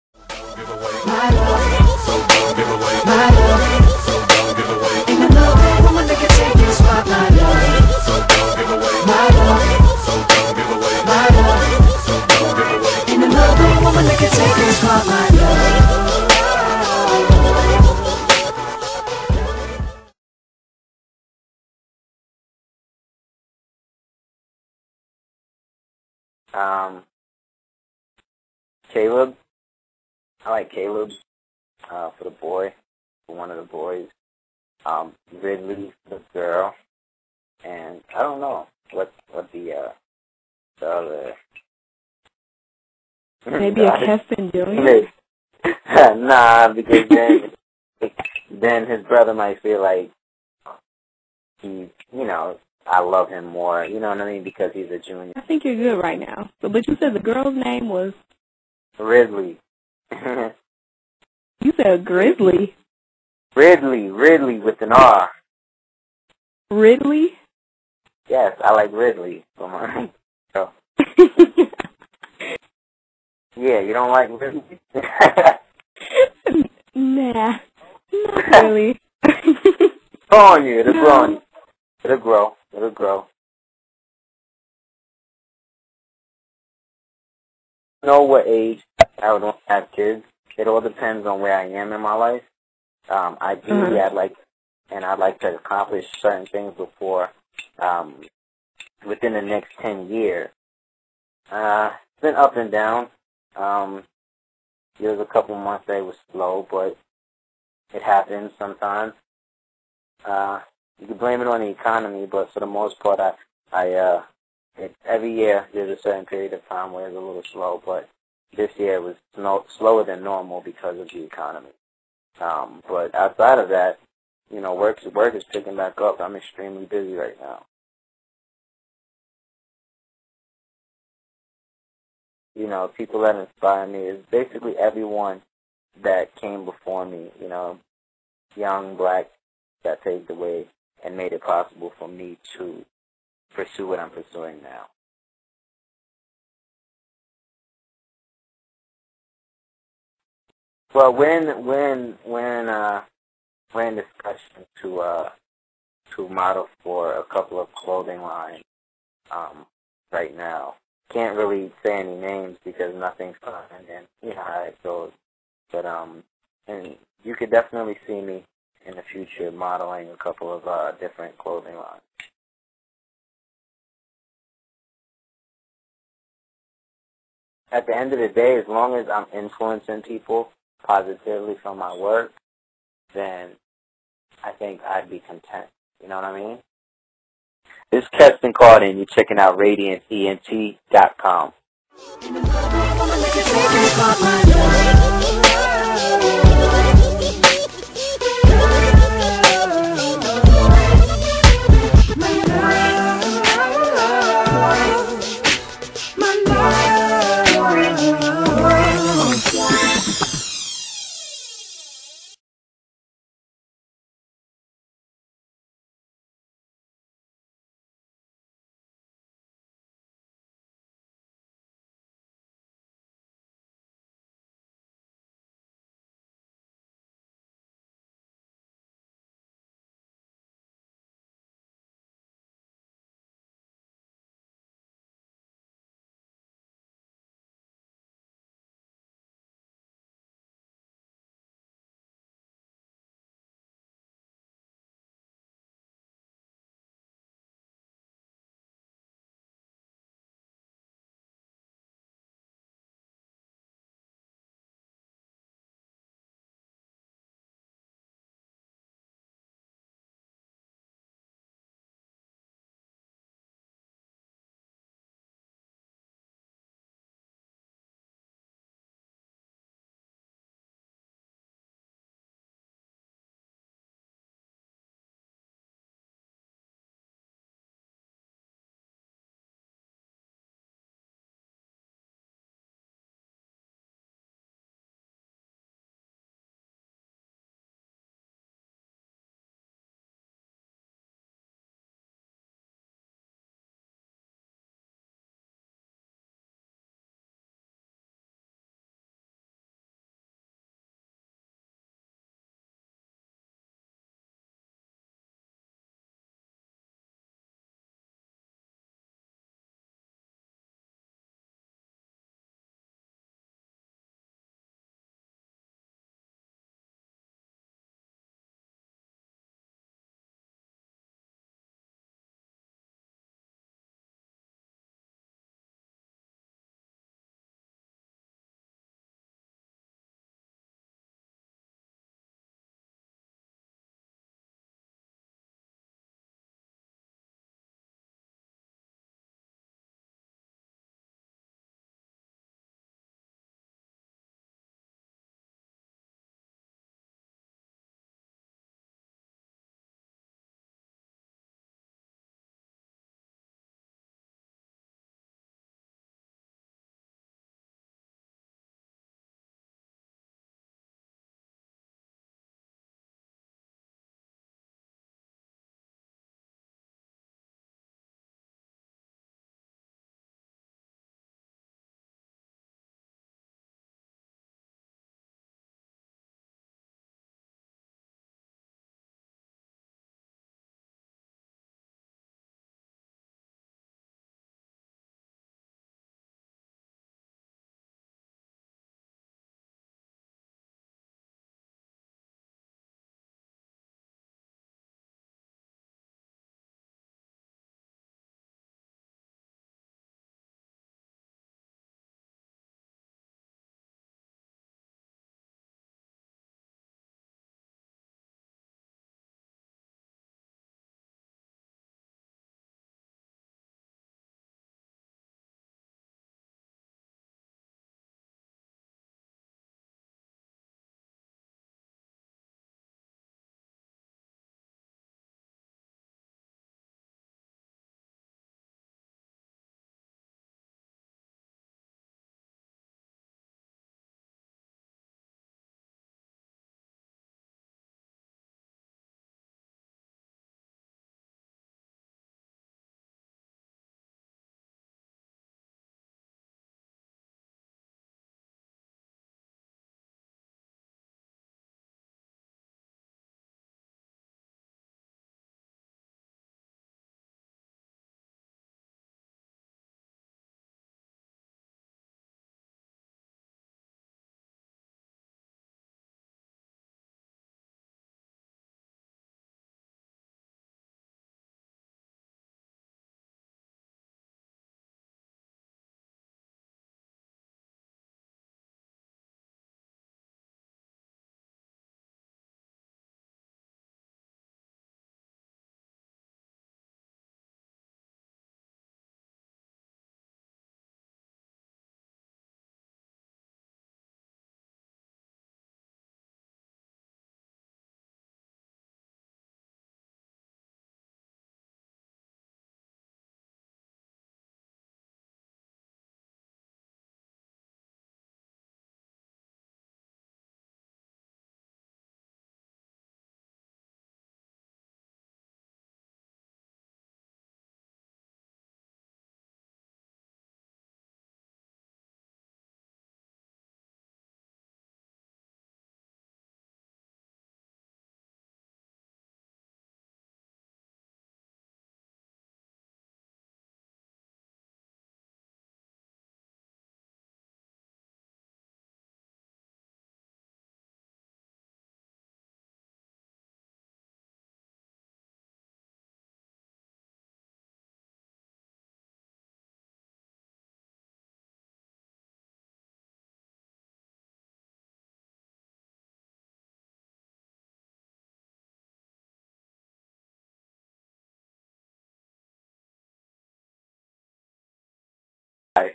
Keston Karter Interview Part 2
Part 2 of Radiance Entertainment's interview with actor/model Keston Karter.